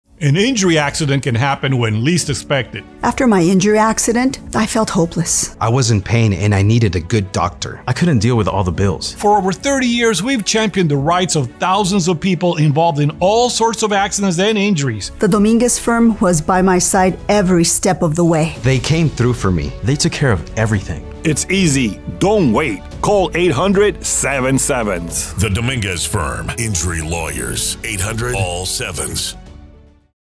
The Dominguez Firm: 30+ Years of Championing the Rights of the Injured
You don’t have to deal with medical bills, lost wages and the stress of dealing with insurance companies by yourself. Let The Dominguez Firm take care of everything so you can concentrate on your recovery.

And if there is no recovery, there is no fee!

-- If there is no recovery, there are no fees or costs.
Attorney J.J. Dominguez